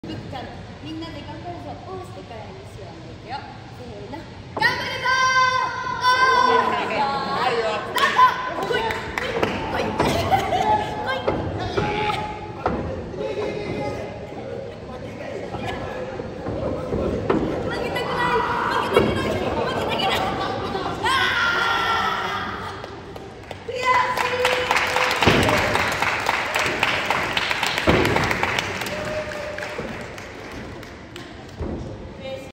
プロレスラー二人 対 ちびっこ達
2024年6月8日　第５回２AW千葉コミュニティプロレス　　幕張コミュニティセンター